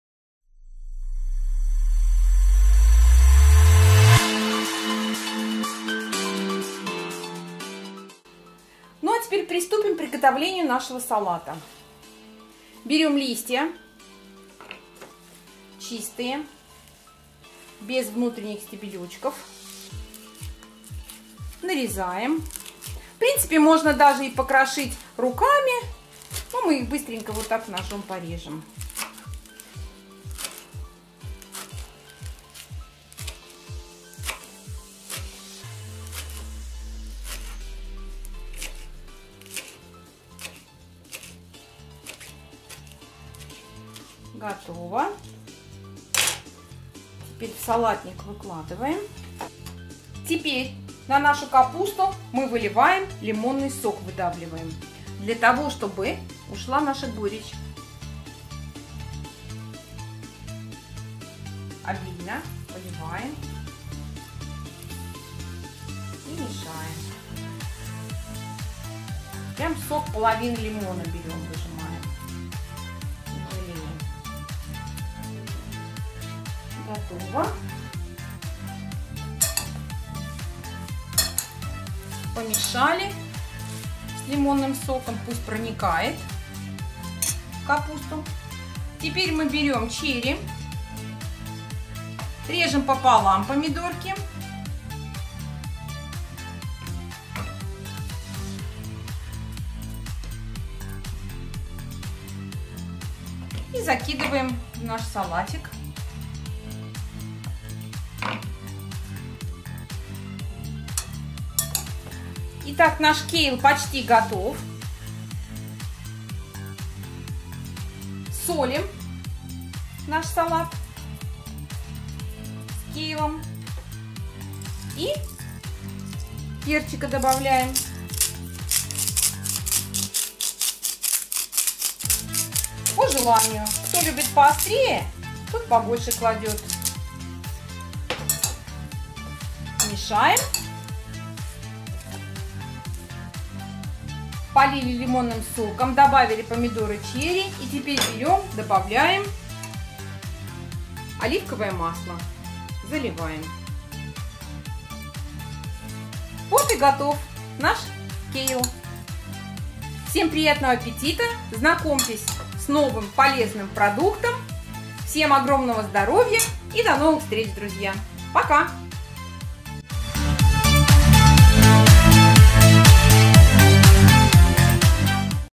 Салат с капустой кейл. ВКУСНО и ПОЛЕЗНО!!!
Сегодня мы готовим замечательный салат с капустой кейл!
 Режем, кейл, затем добавляем черри, посыпаем луком-пореем, сбрызгиваем микс оливковым маслом, выжимаем сок лимона, придерживая косточки, и приправляем солью и перцем. Аккуратно перемешиваем салат. Приятного аппетита!!!